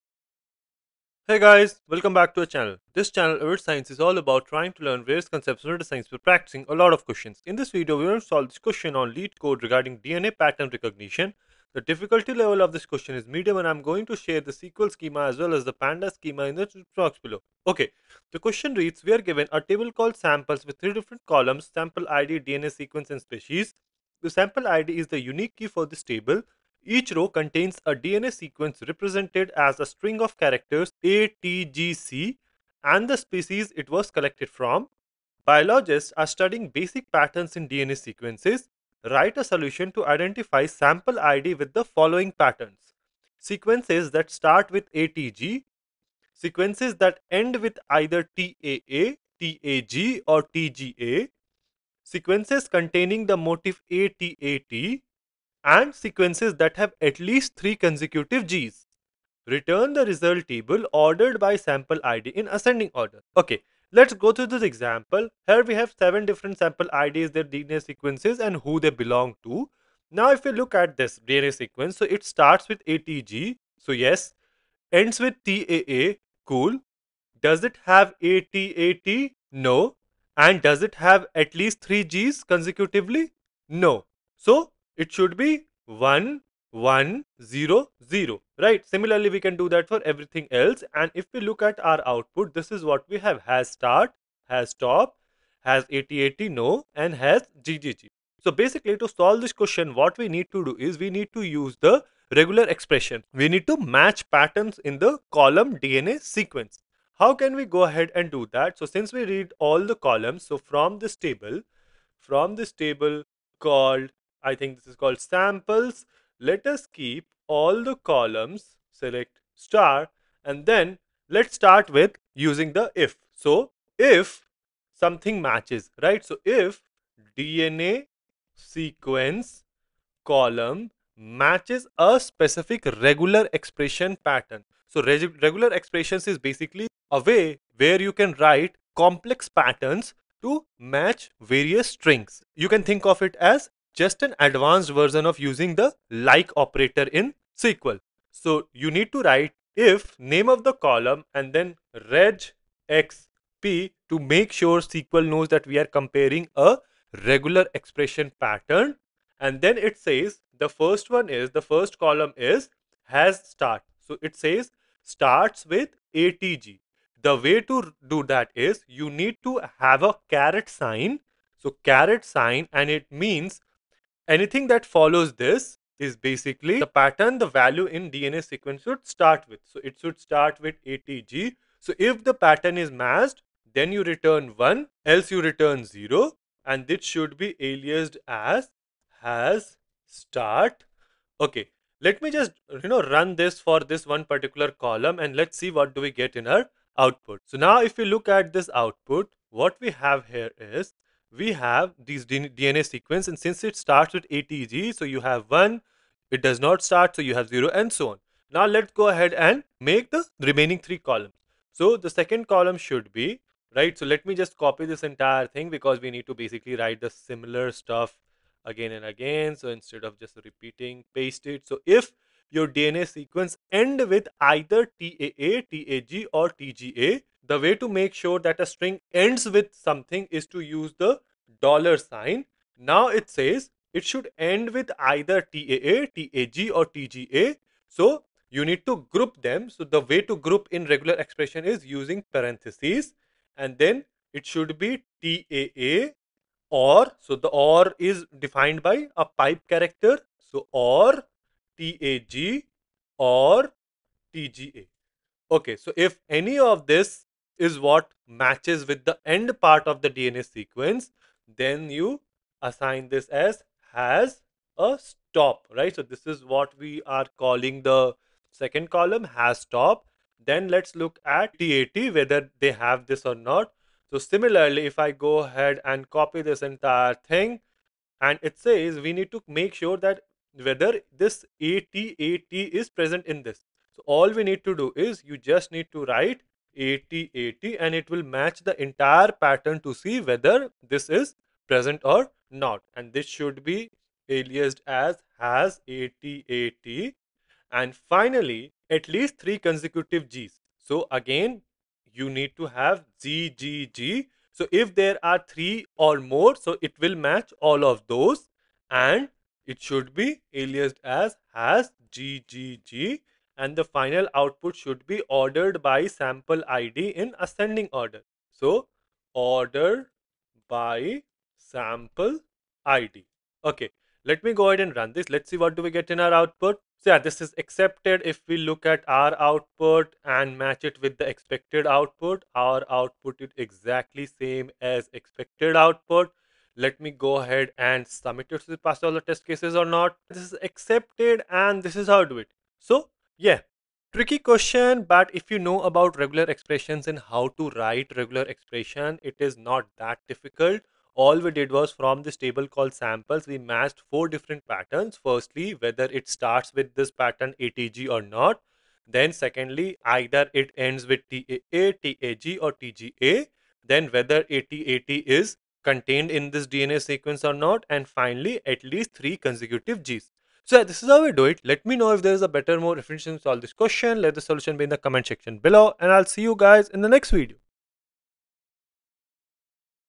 Leetcode MEDIUM 3475 - Using REGEX in SQL - DNA Pattern Recognition | Everyday Data Science
SQL Schema:
CREATE TABLE if not exists Samples (
    sample_id INT,
    dna_sequence VARCHAR(255),
    species VARCHAR(100)
Truncate table Samples
insert into Samples (sample_id, dna_sequence, species) values ('1', 'ATGCTAGCTAGCTAA', 'Human')
insert into Samples (sample_id, dna_sequence, species) values ('2', 'GGGTCAATCATC', 'Human')
insert into Samples (sample_id, dna_sequence, species) values ('3', 'ATATATCGTAGCTA', 'Human')
insert into Samples (sample_id, dna_sequence, species) values ('4', 'ATGGGGTCATCATAA', 'Mouse')
insert into Samples (sample_id, dna_sequence, species) values ('5', 'TCAGTCAGTCAG', 'Mouse')
insert into Samples (sample_id, dna_sequence, species) values ('6', 'ATATCGCGCTAG', 'Zebrafish')
insert into Samples (sample_id, dna_sequence, species) values ('7', 'CGTATGCGTCGTA', 'Zebrafish')

Pandas Schema:
samples = pd.DataFrame({
    'sample_id': pd.Series(dtype='int'),        # Equivalent to SERIAL/INTEGER
    'dna_sequence': pd.Series(dtype='string'),  # Equivalent to TEXT/VARCHAR
    'species': pd.Series(dtype='string')        # Equivalent to VARCHAR(100)